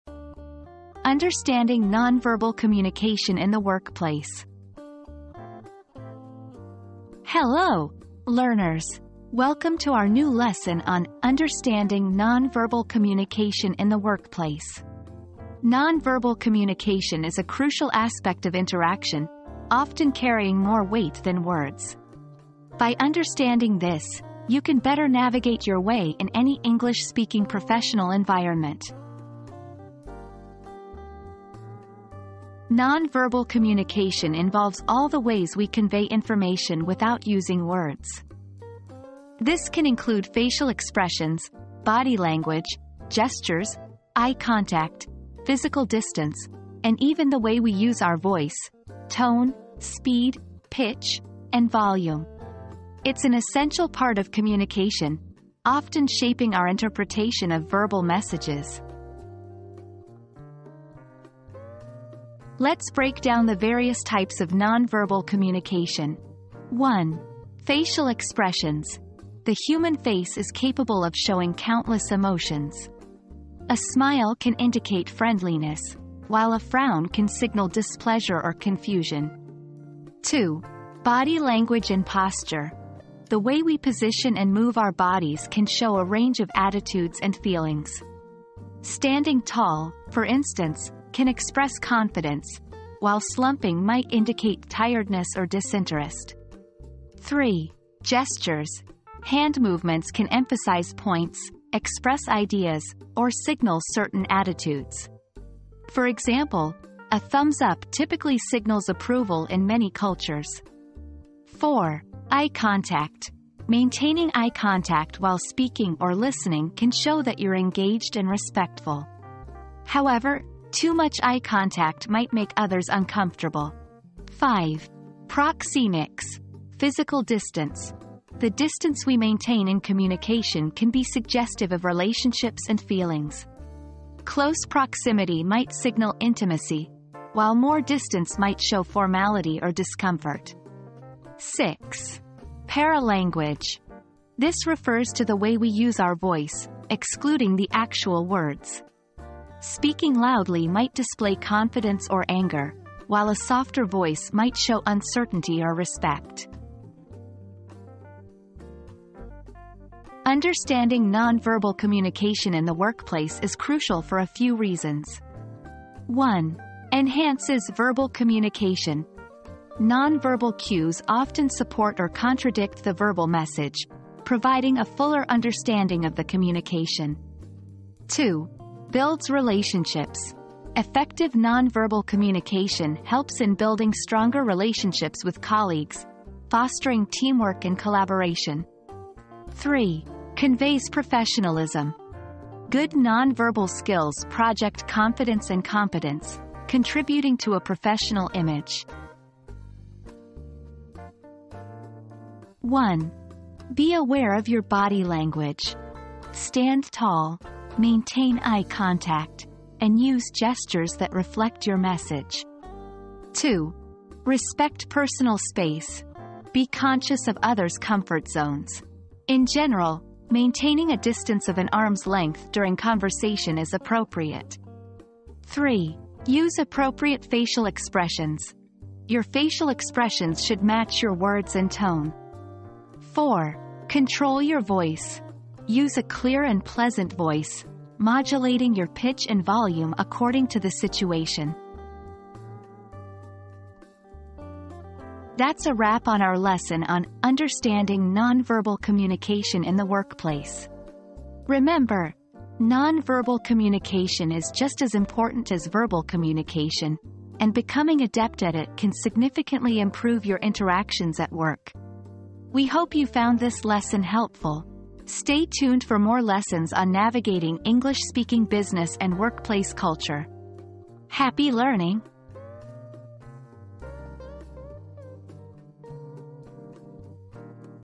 Understanding Non-Verbal Communication in the Workplace
Unlocking the Power of Non-Verbal Communication at Work • Discover the secrets of non-verbal communication and how it can shape your success in the workplace. From facial expressions to body language, learn how to navigate any professional environment with confidence and understanding.

00:00 • Introduction - Understanding Non-Verbal Communication in the Workplace
00:32 • What is Non-Verbal Communication?
01:02 • Types of Non-Verbal Communication
02:39 • Importance of Non-Verbal Communication in the Workplace
03:22 • Tips for Effective Non-Verbal Communication in the Workplace